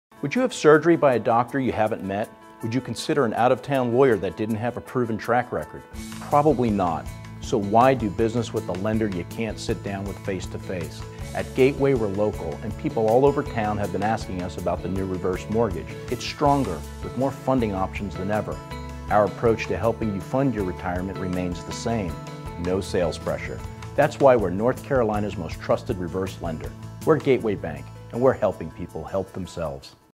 Gateway Bank Mortgage - Reverse Mortgage Assistance in Raleigh, NC
This video is a commercial for Gateway Bank Mortgage's Reverse Division who offers reverse mortgage assistance in Raleigh and throughout North Carolina.

Transcription:

Would you have surgery by a doctor you've never met? Would you consider an out of town lawyer that didn't have a proven track record. Probably not. Then why do business with a lender you can't sit down with face-to-face? At Gateway we're local and people all over town have been asking us about the new reverse mortgage. It's stronger with more funding options than ever. Our approach to helping you fund your retirement remains the same...no sales pressure. That's why we're North Carolina's most trusted reverse lender. We're Gateway Bank and we're helping people help themselves.

Gateway Bank Mortgage, Inc.

NMLSR 46308 | Equal Housing Lender